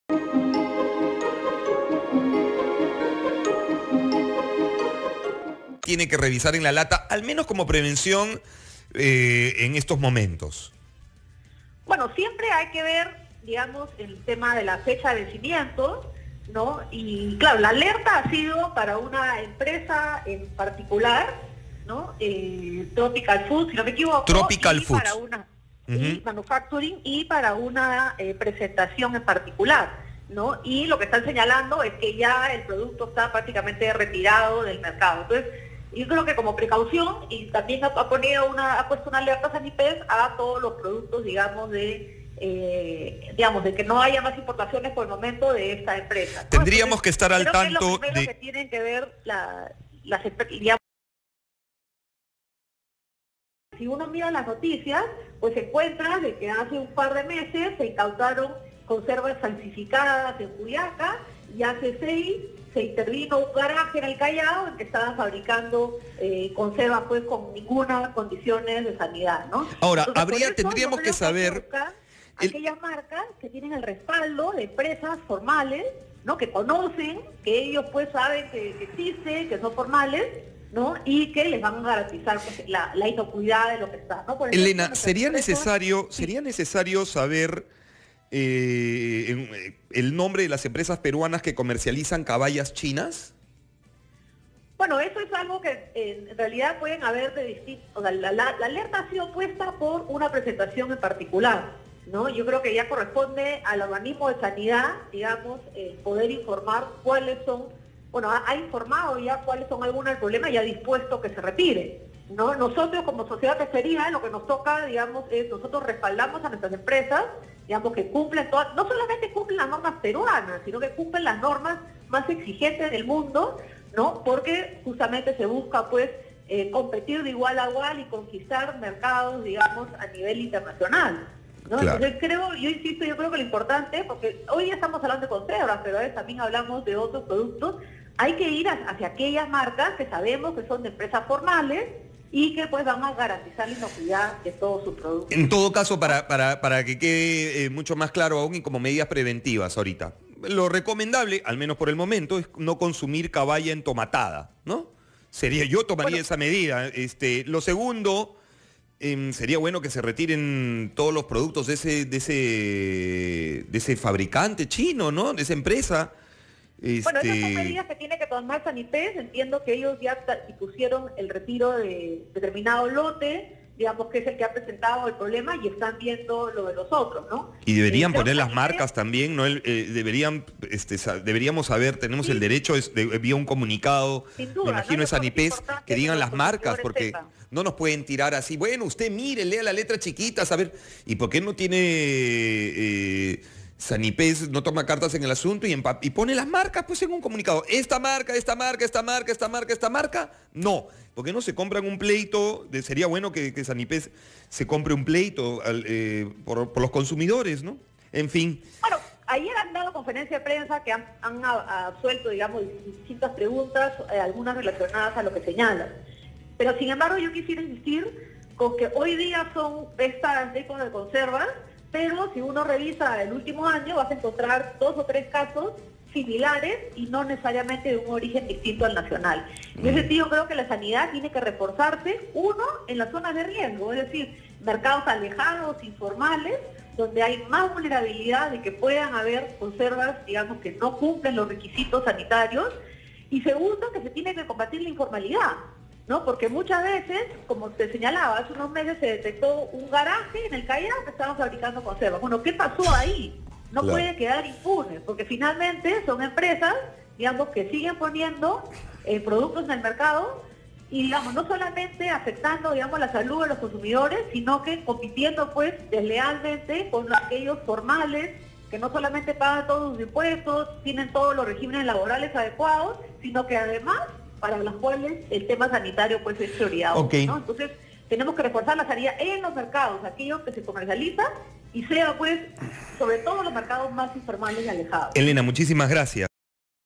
¿Cuáles son los síntomas que provoca consumir conservas contaminadas?
Elena Conterno, presidenta de la SNP, habló sobre la orden de retiro de conservas de caballa. | Capital